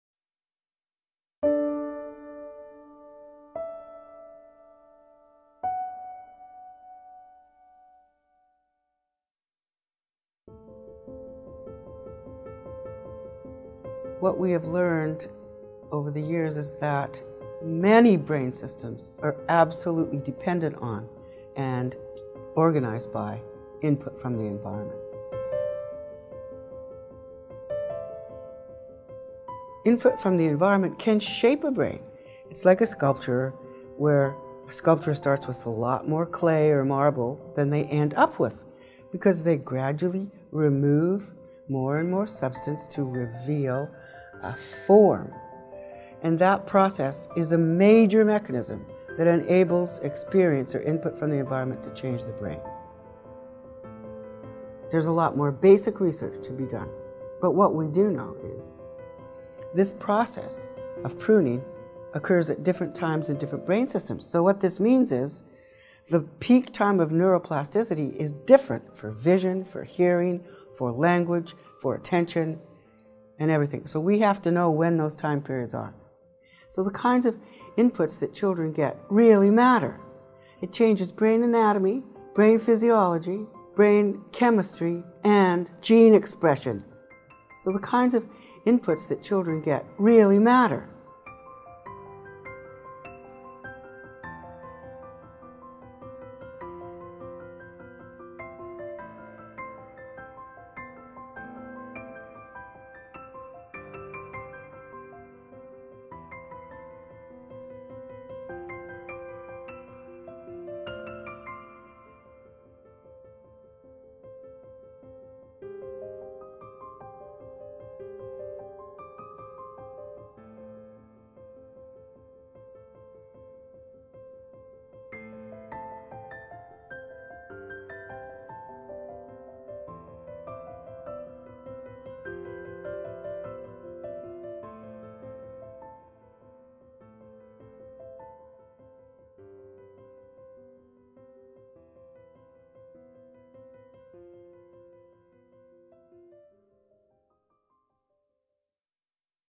Your Child's Brain - What Every Parent and Caregiver Needs to Know
Input From the Environment Shapes the Brain

In children’s brains there is a process of pruning that occurs at different times in different brain systems such as vision, hearing, language, and attention.  It’s a mechanism that shapes the form of the brain, and it depends on input from the environment.
Experience Builds the Brain Through Neuroplasticity

The input that children receive really matters. Environmental input leads to changes in brain physiology, brain anatomy, brain chemistry and gene expression.

Featuring:
Dr. Helen Neville